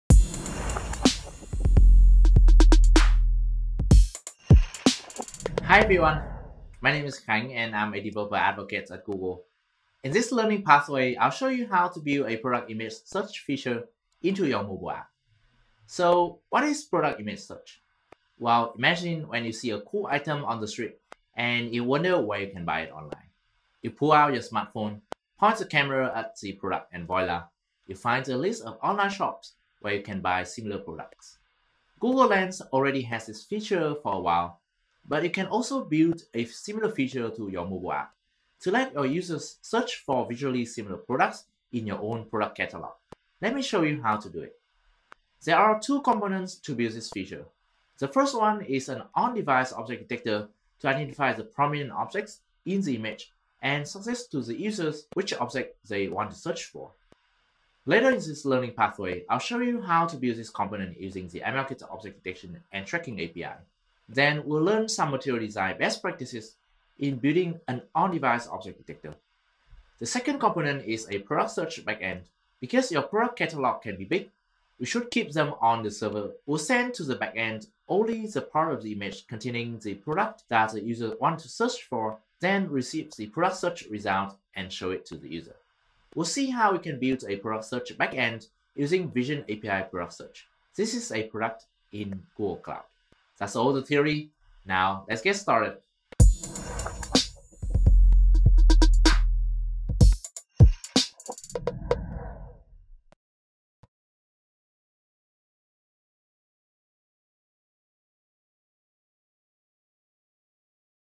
On-device product image search: Introduction
A new learning pathway from Google Developers to help you build On-Device Machine Learning apps. In this pathway, you’ll learn how to build an app for Android that enables your user to search for visually similar products from your product catalog.

The first video in the series introduces the components needed to build a product image search feature: ML Kit and Cloud Vision API Product Search.